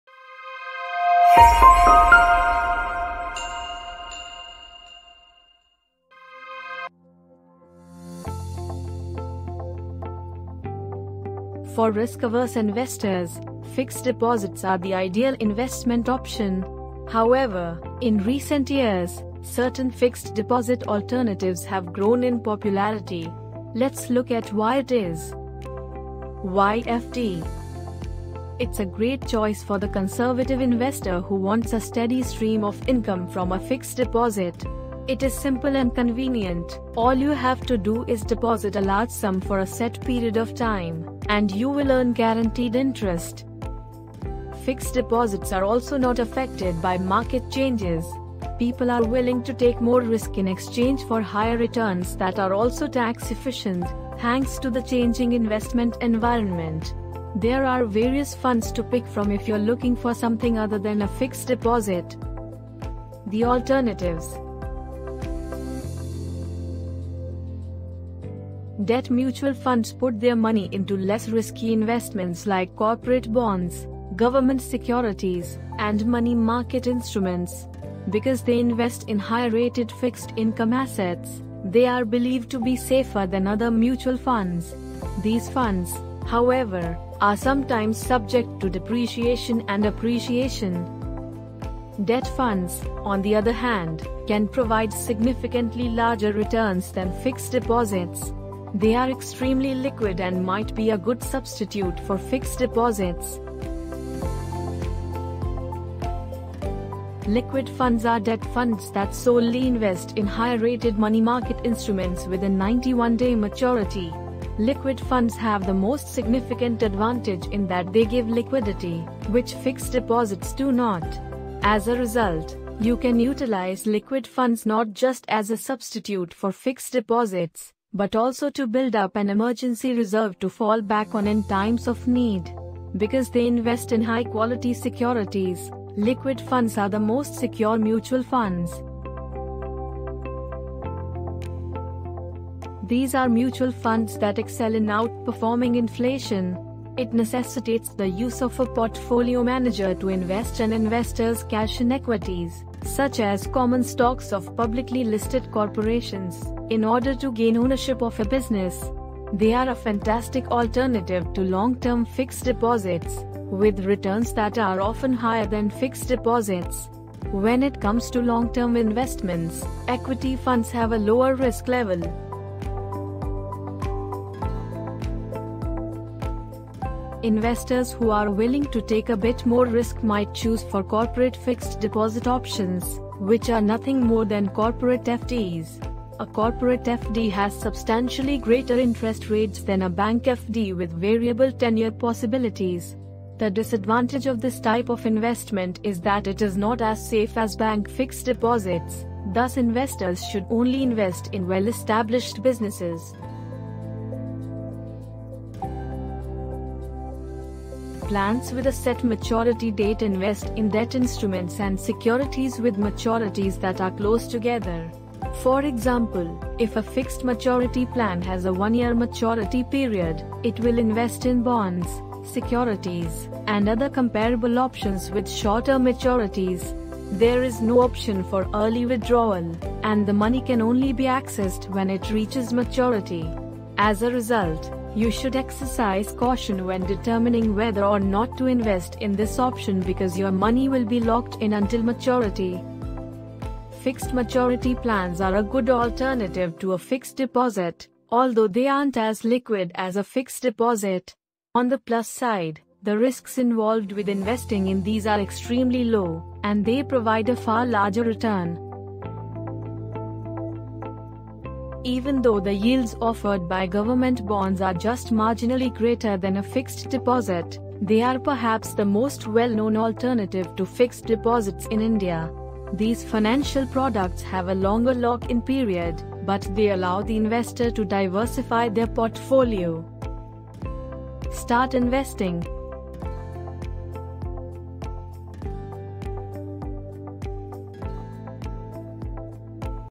Safe Investment Alternatives To Fixed Deposits
For risk-averse investors, fixed deposits are the ideal investment option. However, in recent years, certain fixed deposit alternatives have grown in popularity.

Let's Get Xplained

Your support means a lot for us.